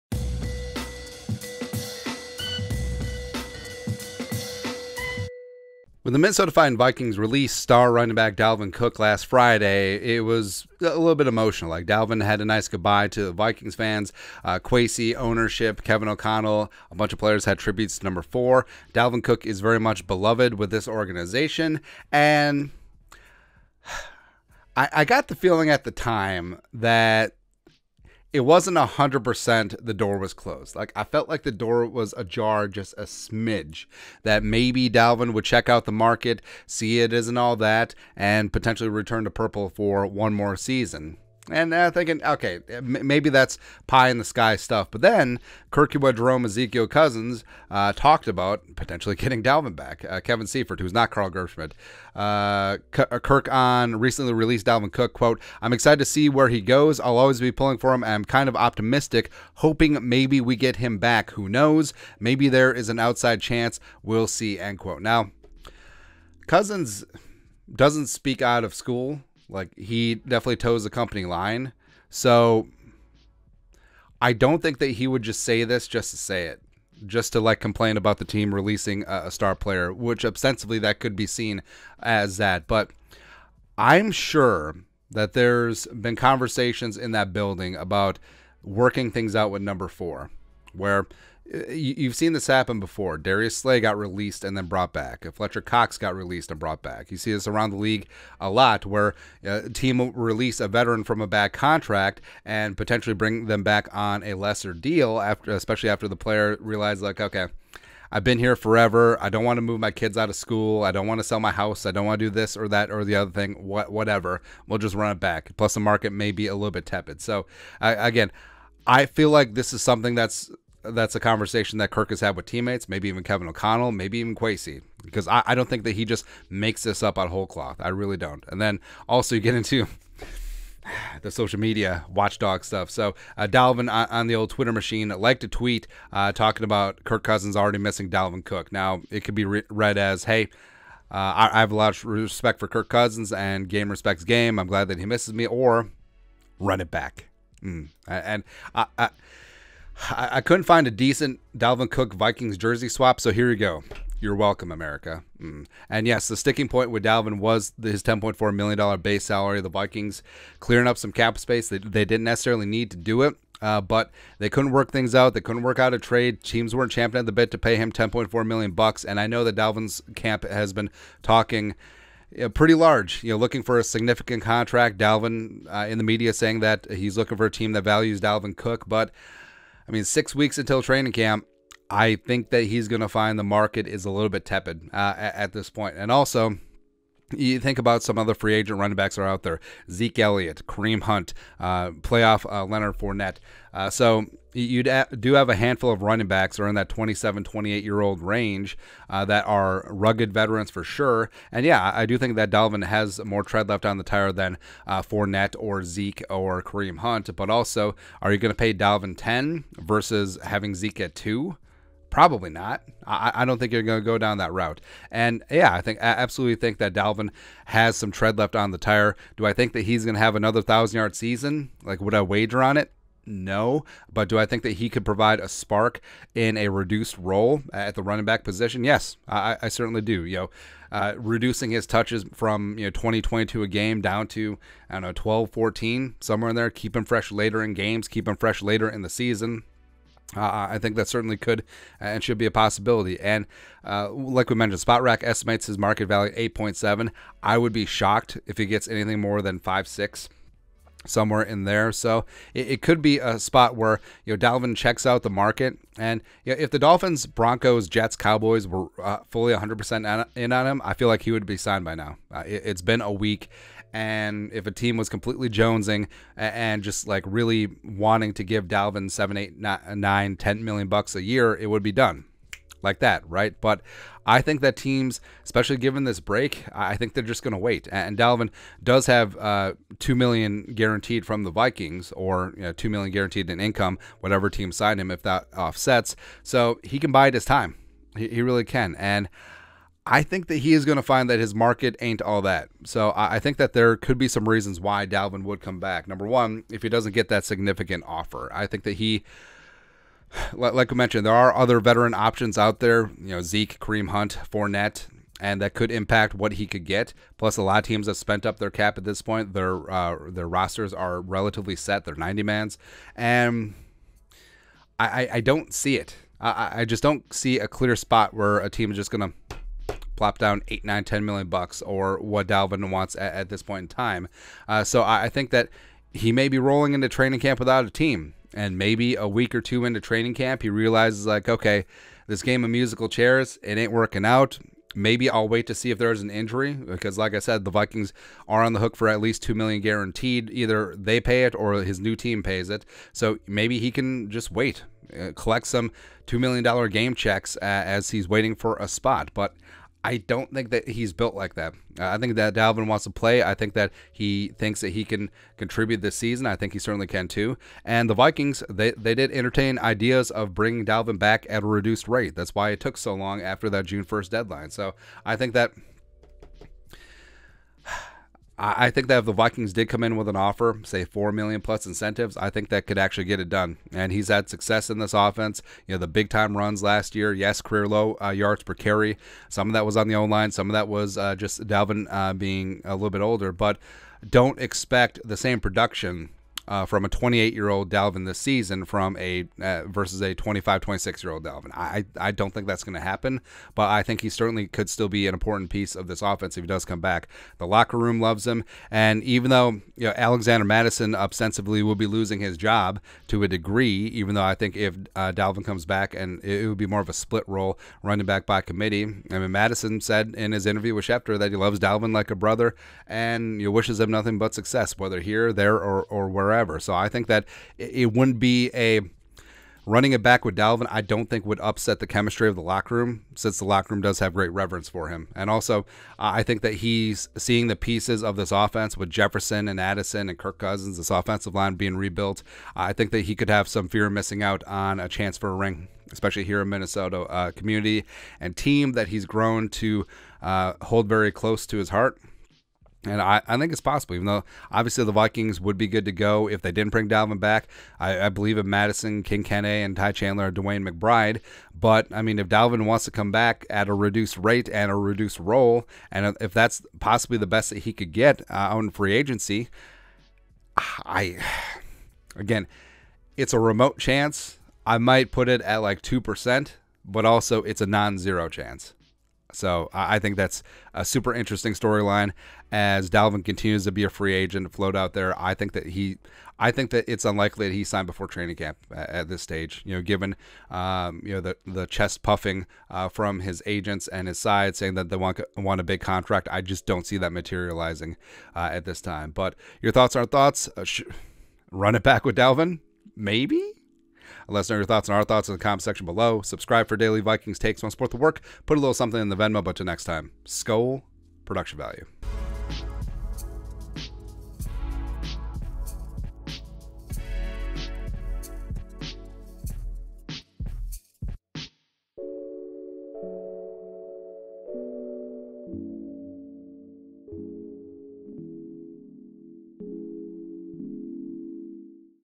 Could Dalvin Cook and the Minnesota Vikings Run it Back? 🤔👀
Could the Minnesota Vikings run it back with recently released running back Dalvin Cook? Never say never.